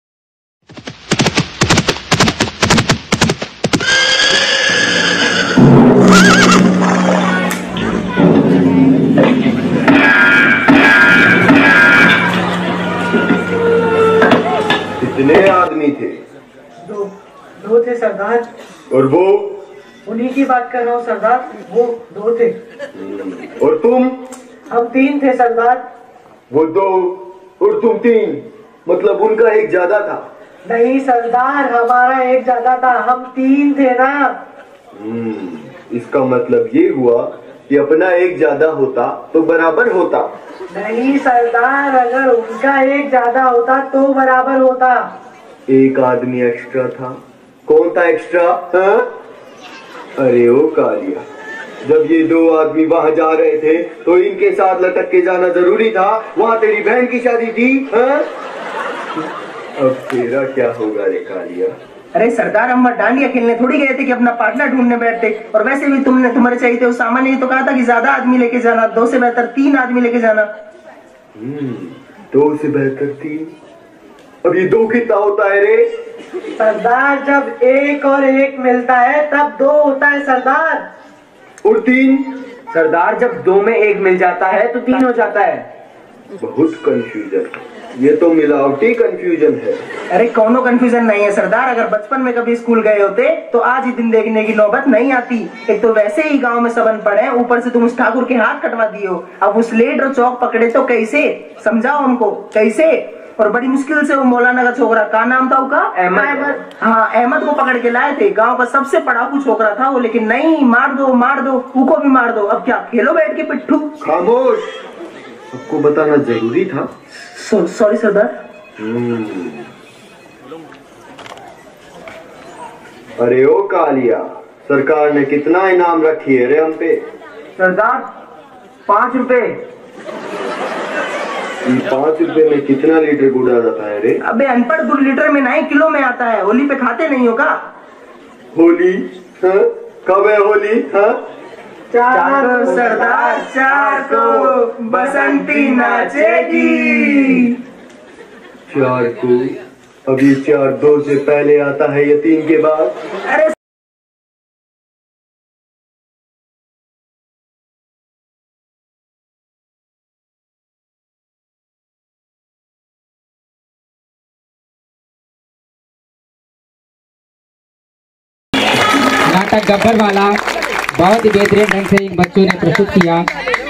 Anpadh Gabbar । अनपड़ गब्बर
अनपढ़ गब्बर नाटक में गब्बर के किरदार में प्रिंस साथ ही सह कलाकार समीर, दिव्यांश, और नेहाल ने संत शिरोमणि गुरु रविदास जयन्ती समारोह में लोगों को जम कर हँसाया